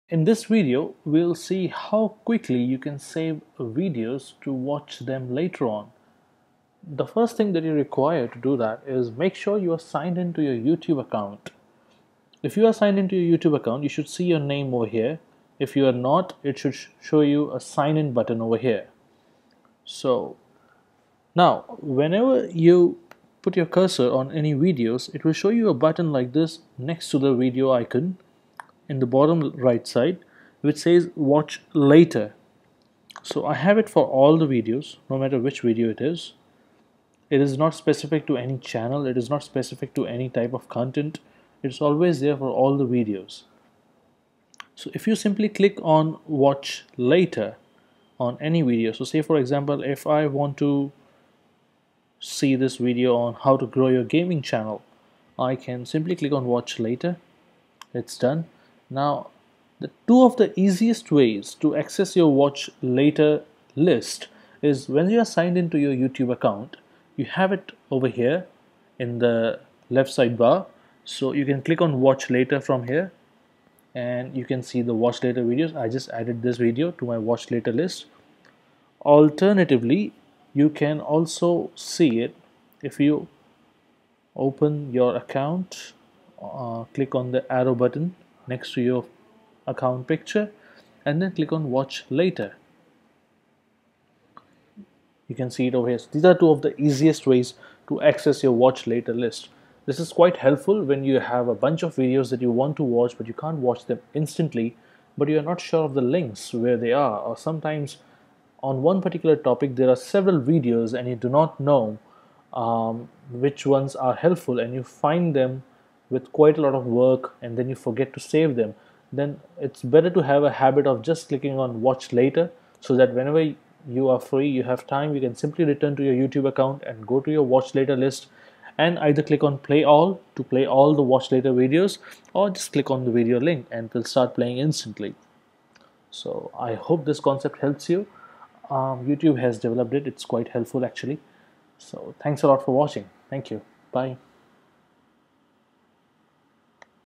Where to find the videos added using "Watch Later" button?
A quick video on how you can find your "Watch Later" videos.